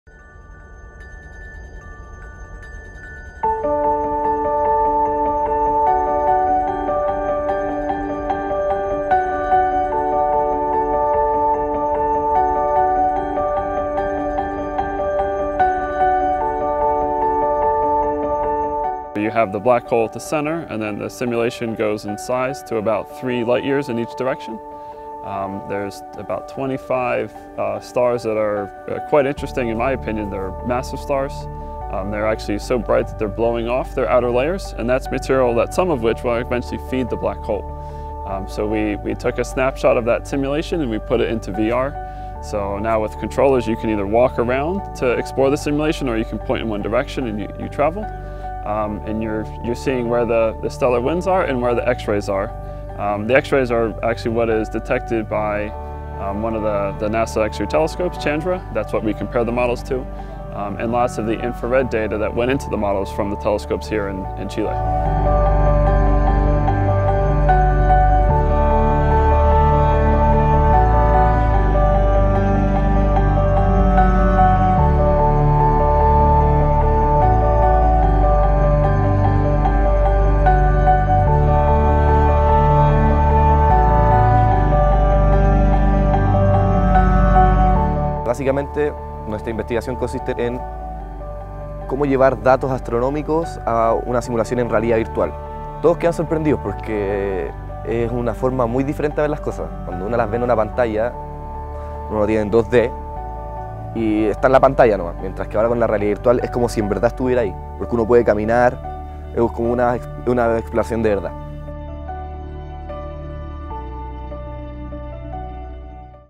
Un paseo virtual por el centro de la galaxia
Una aplicación gratuita de realidad virtual desarrollada por astrofísicos UC permite recorrer el centro de nuestra galaxia y ver el agujero negro que está en su centro.